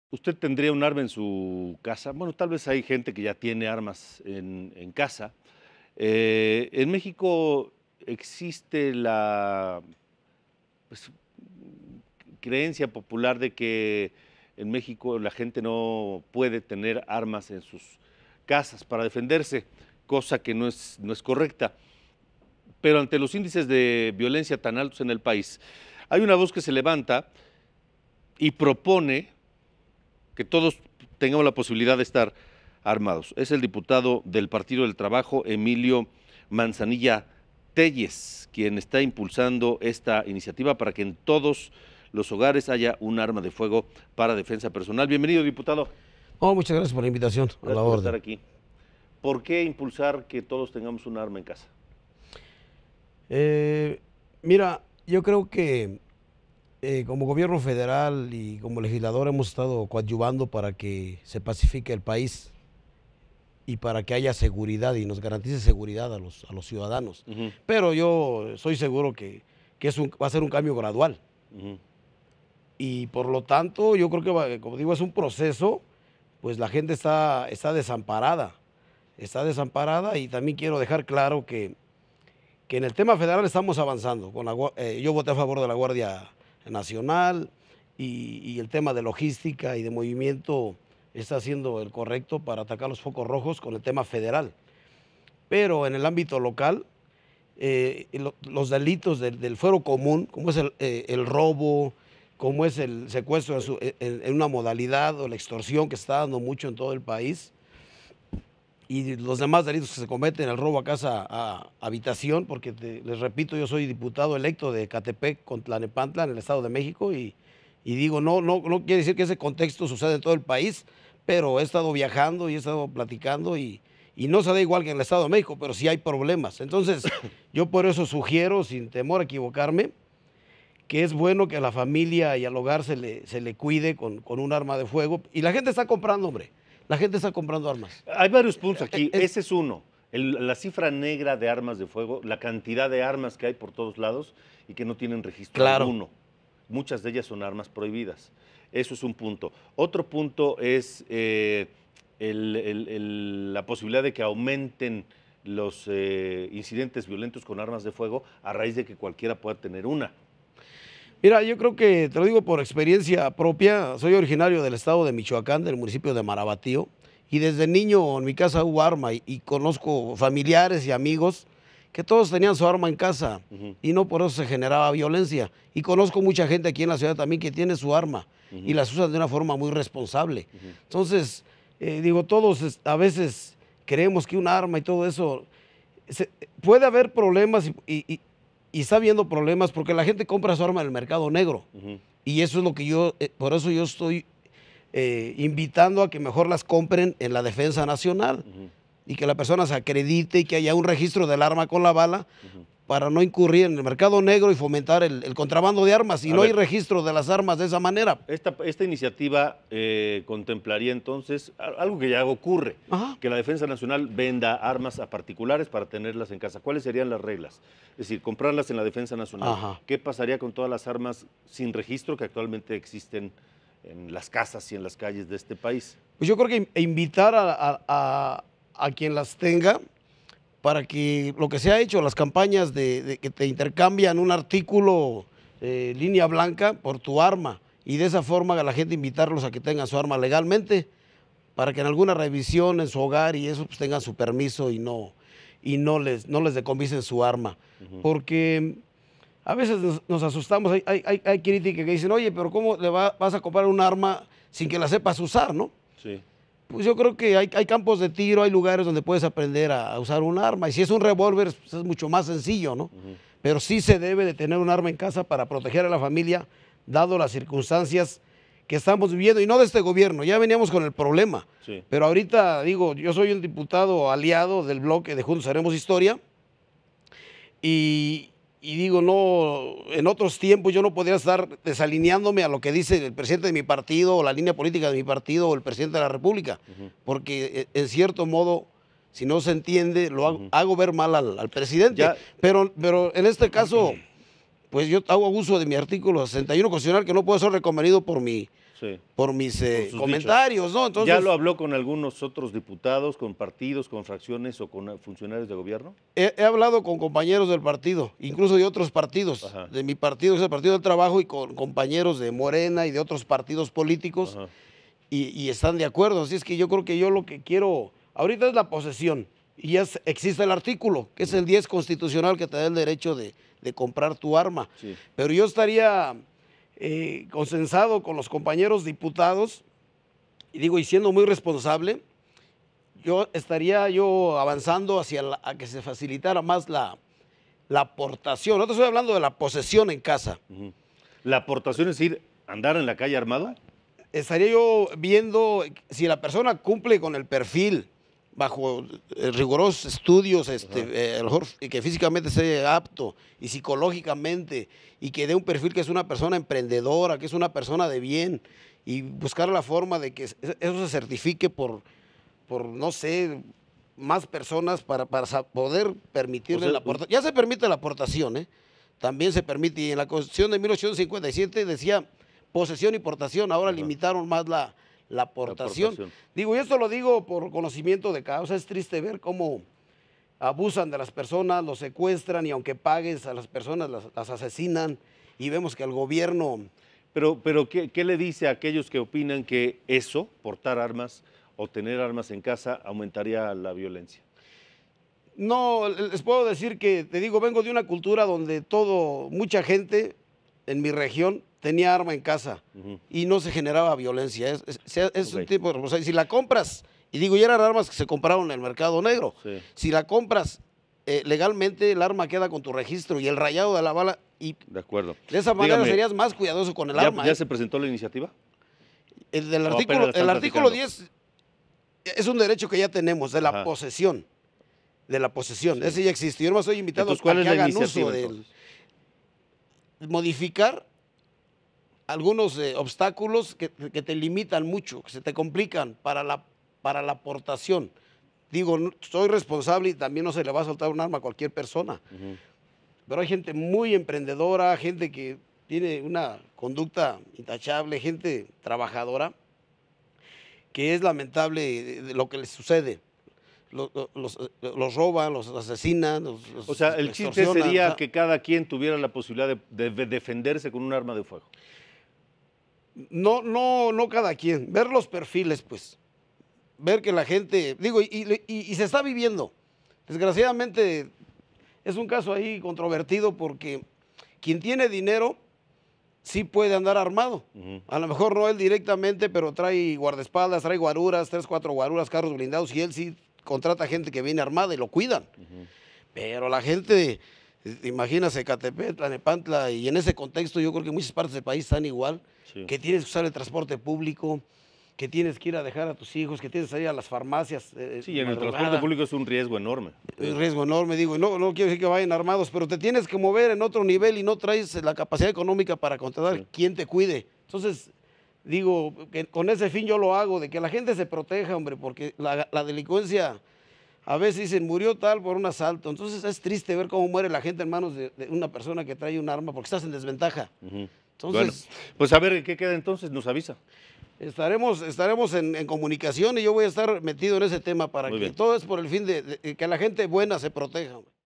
Mexicanos necesitan armas hasta que haya seguridad, afirma diputado
Los resultados de la lucha contra el crimen se darán de forma gradual, pero mientras esto sucede la sociedad está desprotegida, así que es necesario que los mexicanos compren armas para estar protegidos ante la delincuencia, así lo aseguró Emilio Manzanilla, diputado del Partido del Trabajo.